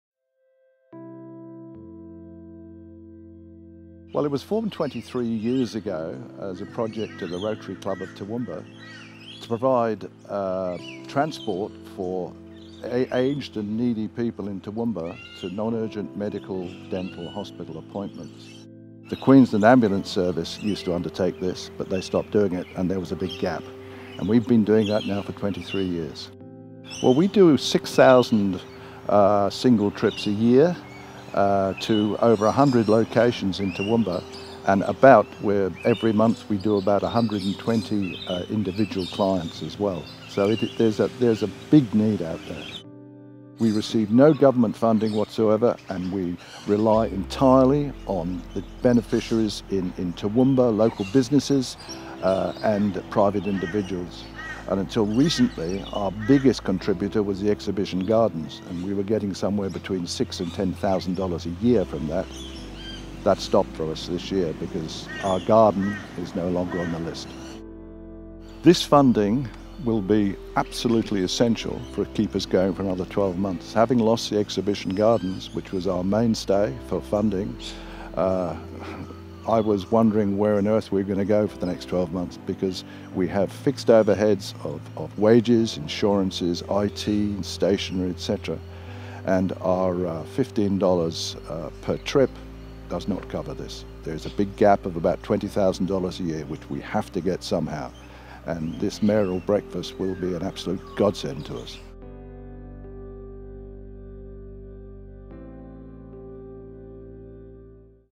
Toowoomba Community Care Transport Inc - Mayoral Prayer Breakfast
Toowoomba Community Care
Transport Inc (TCCT) volunteers
have traveled thousands of
kilometers to ferry people to and
from appointments in the 23 years
since the service was started by
the Rotary Club of Toowoomba.
The organisation was chosen as
the charity beneficiary for this
year’s Mayoral Prayer Breakfast.